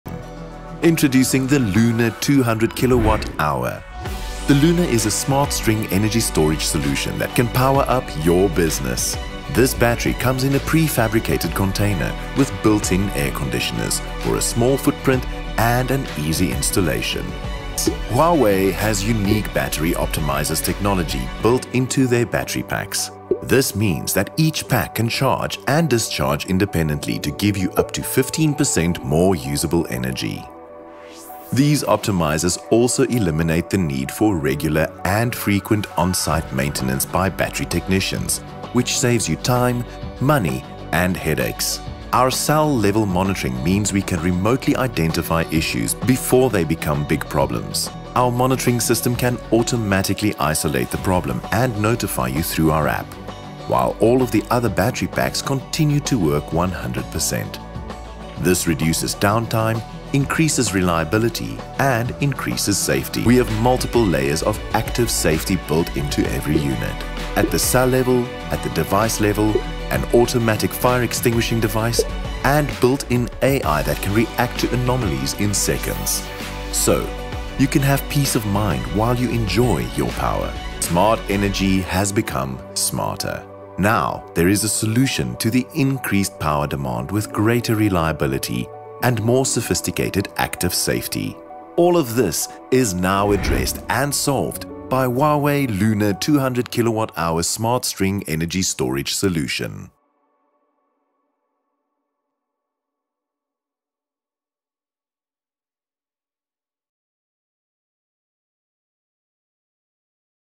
HUAWEI Smart String Energy Storage System - LUNA2000 | 200kWh
The Huawei Luna2000 is available at GC Solar.
GC Solar is an authorized distributor of Huawei. Huawei's Smart String ESS is a reliable and safe energy storage system that provides multiple benefits for businesses.

Safety Features:
Real-time battery cell detection for early warnings and rapid shutdown to prevent fire.
Active fire protection system with automatic gas relief.
Industry-leading explosion relief venting for personnel safety.
Reliable Power Supply:
Helps save on electricity bills, reduce carbon footprint, and overcome blackouts.
Intelligent grid forming for enhanced voltage and frequency stability.
Energy Optimization:
Built-in optimizer manages each battery pack independently for maximum energy generation.
Three working modes (Max. Self-consumption, TOU, and Peak Shaving) for automatic and intelligent power control.
Integrates with SmartEMO to achieve 10% more energy.
Lower Costs:
Automatic SOC synchronization after battery replacement.
Remote faulty module detection reduces maintenance costs.
Multiple Battery Options:
Various capacities to satisfy diverse scenarios.
97 kWh battery for optimal benefits even for small industrial applications.